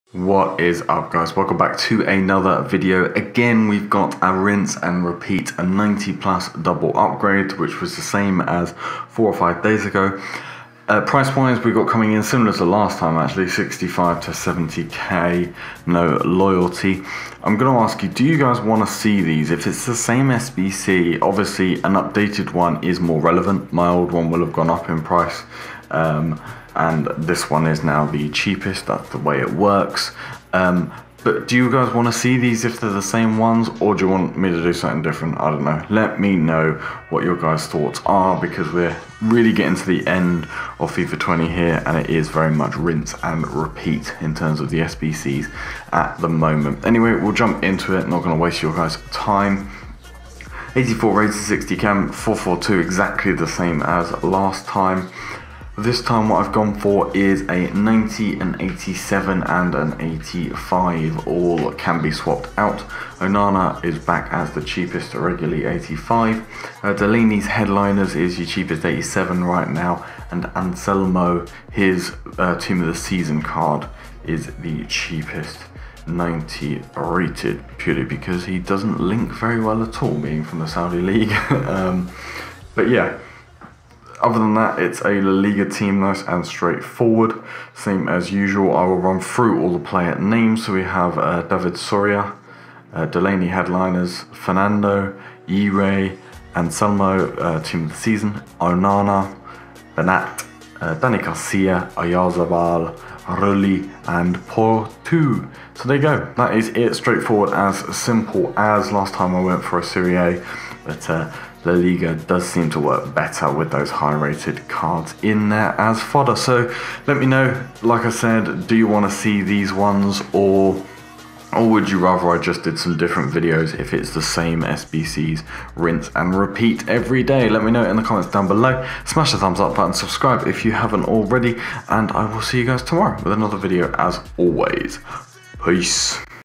90+ Double Upgrade SBC Cheapest Solution!!
31st August Version
How to complete a squad for a pack with two players rated 90 OVR or higher! NO LOYALTY!